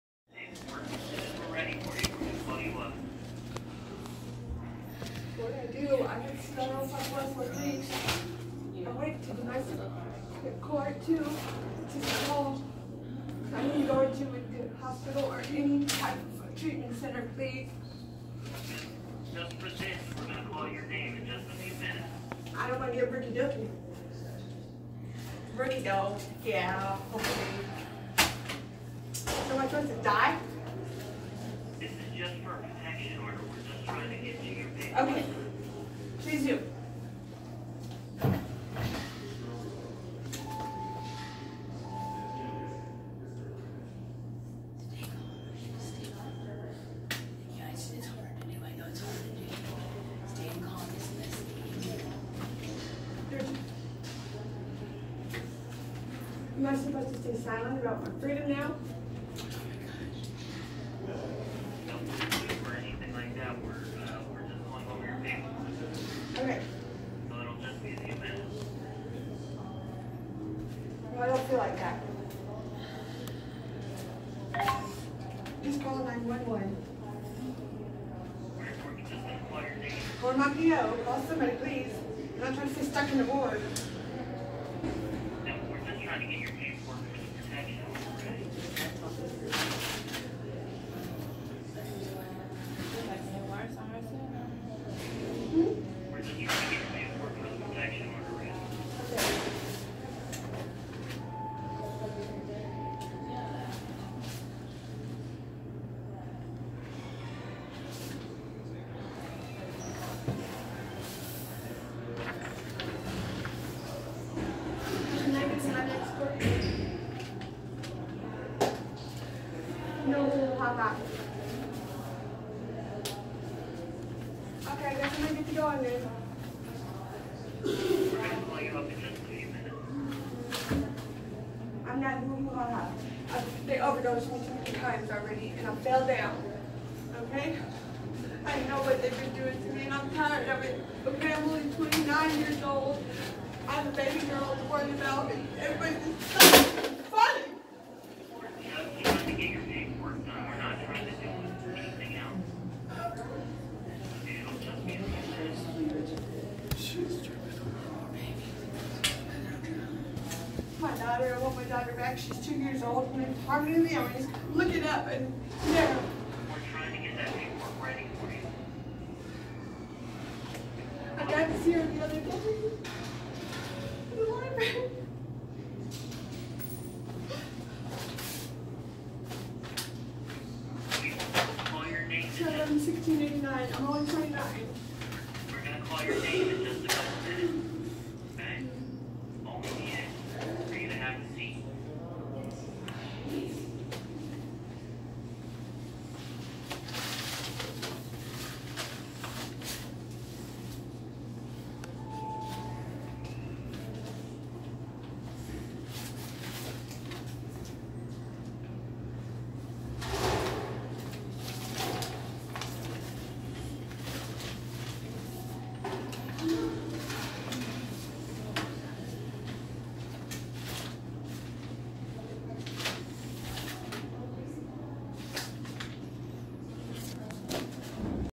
Crazy methhead loses her mind
Young lady in the courthouse loses her mind while filling out paper work